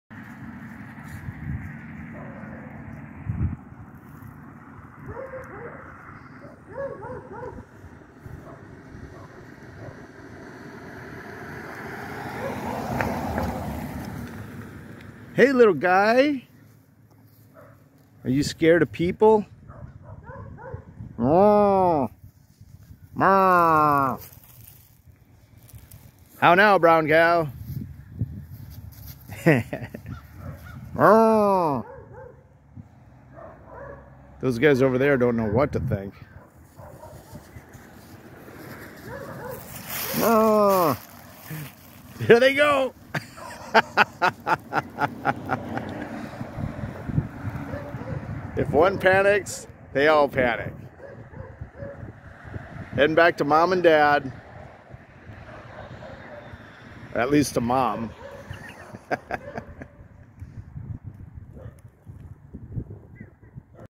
Cute Baby Brown Calfs Run!! - How to annoy a cow into fleeing.
Yeah- not the reaction I was hoping for - thought they may wander over - ha.